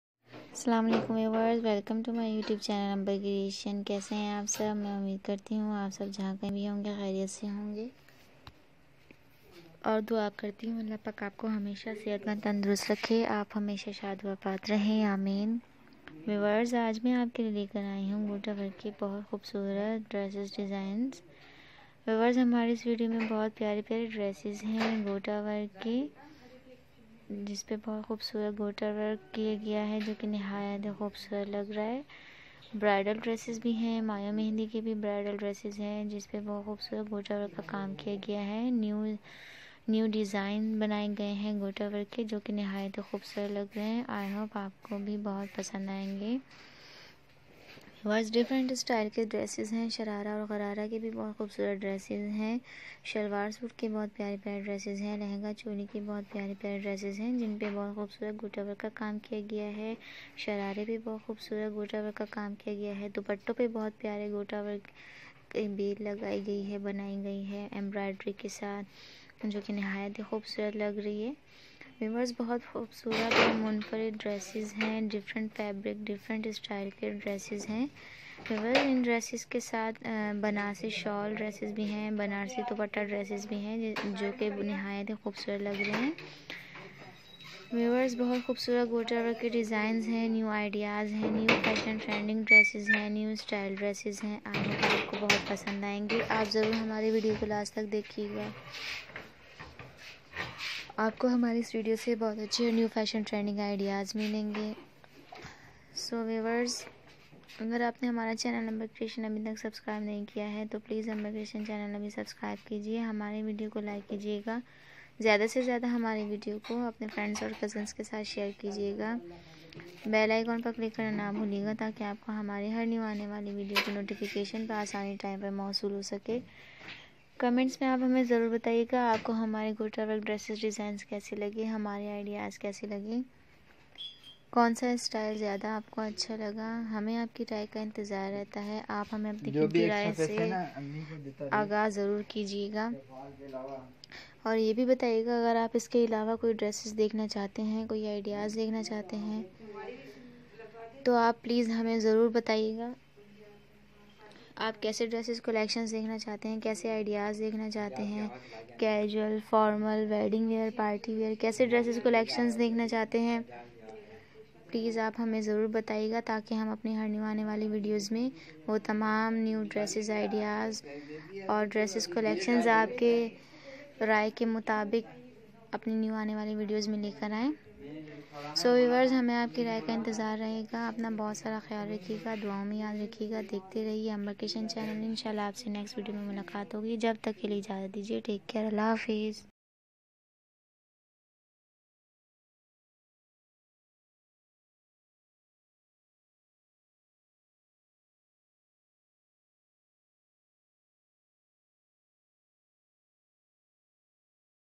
Very Attractive & Stylish Mayon Mehndi Sharara Gharara Gotta Work Dresses Designs Ideas
Is video me ap dakhe ge very attractive & stylish mayon mehndi sharara gharara gotta work dresses designs ideas